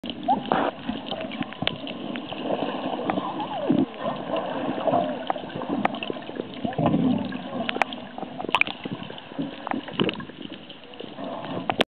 Shark Cage diving 4
Shark Cage Diving with Brian McFarlane off Gansbaai, South Africa, July 2010 - Clip 3.  Footage shot by Bart.